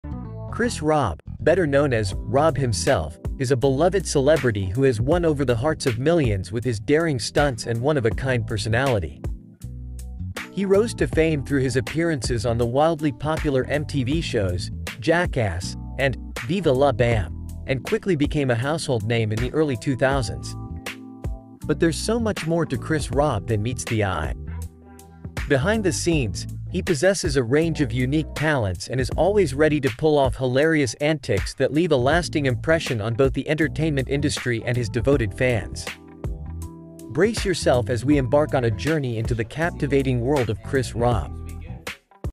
Chris Raab | Legend Of Love
hris Raab, also known as “Raab Himself,” is a celebrity who captured the hearts of millions through his outrageous stunts and quirky personality. Known for his appearances on the popular MTV series “Jackass” and “Viva La Bam,” Raab quickly became a household name in the early 2000s.